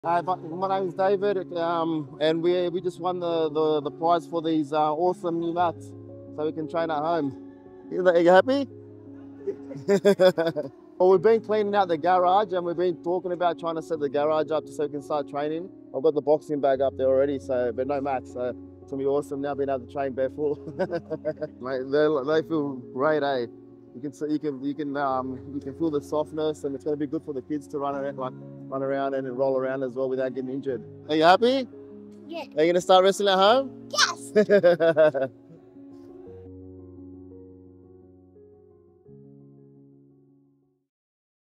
Dave and his adorable little daughter won our raffle and took home the Evolution Mat Kit
We were amazed to witness a truly special moment at the NSW BJJ Winter Cup!

Dave and his adorable little daughter won our raffle and took home the Evolution Mat Kit—a perfect setup for training at home.

Seeing the pure joy on his daughter’s face, knowing they now have mats to train together, was simply priceless. It’s moments like these that remind us why we do what we do.

Congratulations, Dave! We can’t wait to see the amazing memories you’ll create on your new mats.